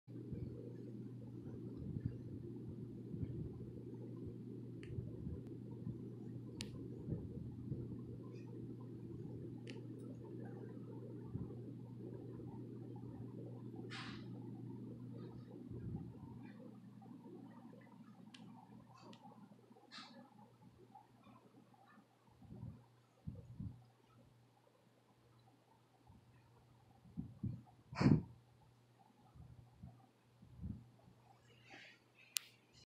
Ezio the Galgo just wants love 💚
I recently adopted a galgo from Spain in April, 2019. I adopted him through Ibizan Hound Rescue. I highly recommend anyone looking to add a dog to the family to consider adopting a galgo.